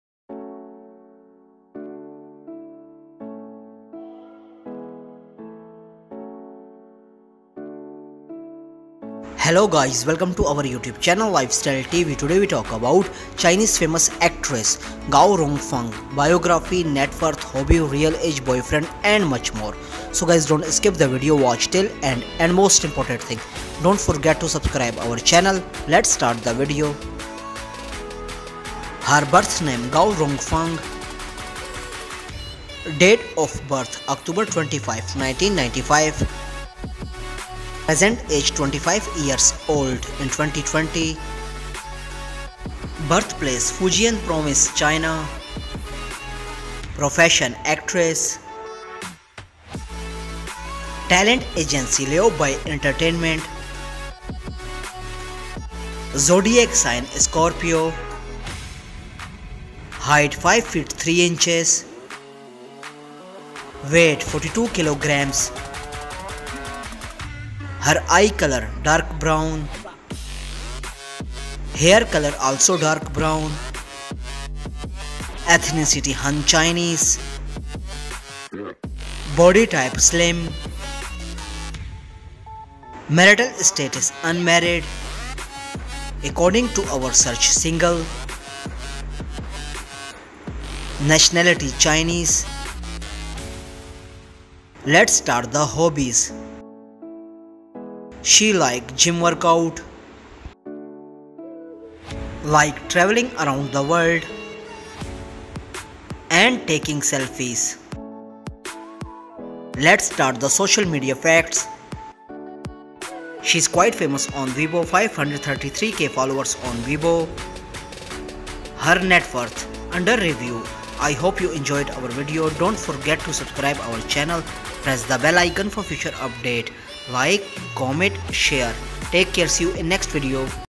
Gao Rong Fang Lifestyle (Uncle Cool) Biography, Boyfriend, Income, Real Age, Height, Weight, Facts
Fanny Gao Rongfang Lifestyle 2021 (The Investigator) Biography, Boyfriend, Income, Real Age, Height, Weight, Facts

Gao Rong Fang (English name: Fanny) is a Chinese actress and former Ruili model. In December 2016, she participated in the filming of the LeTV web series, "The Distance Between Me and You Light Years". She officially debuted in the entertainment industry in 2017 after signing a contract and participated in the filming of Sohu.com’s series, "Dear Brain".

Turkish Translated